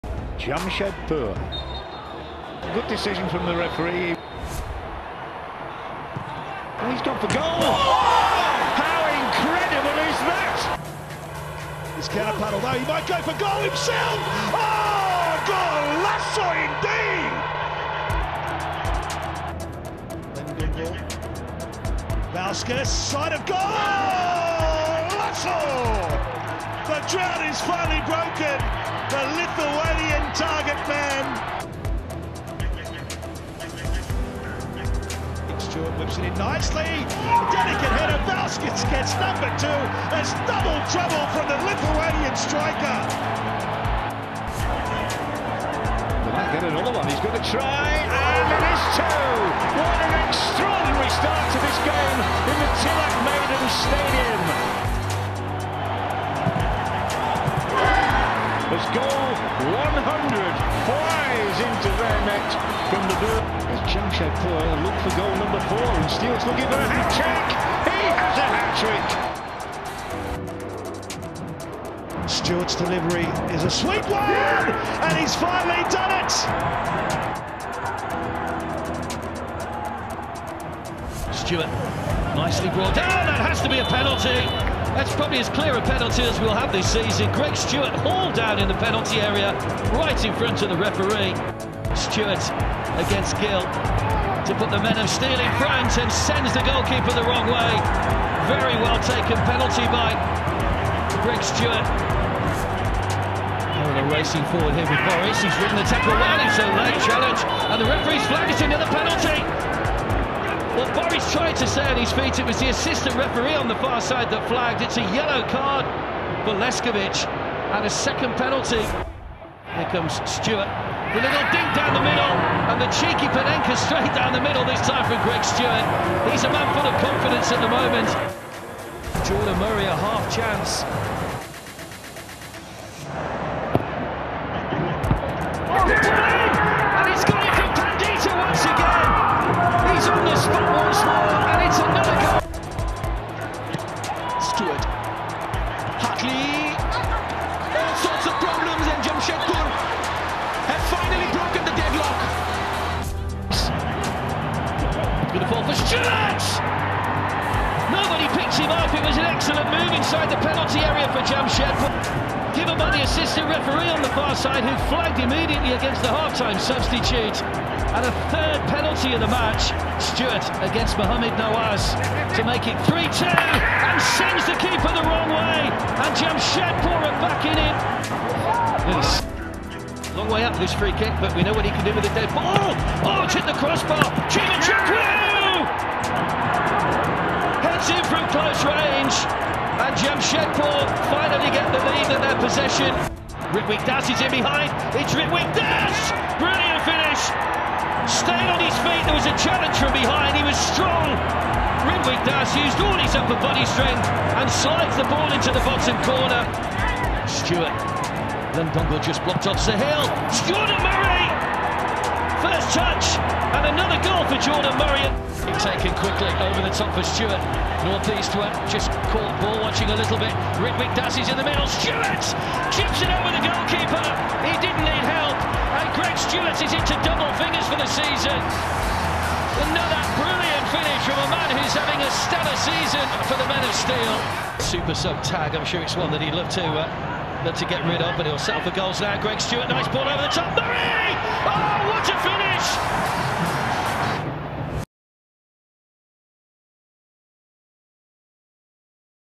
Greg Stewart: Goals and assists by the Player of the Season | Jamshedpur FC | ISL 2021-22
Greg Stewart scored and assisted 10 goals each and became the ISL Player of the Season.

Watch the Jamshedpur FC forward tear defences apart.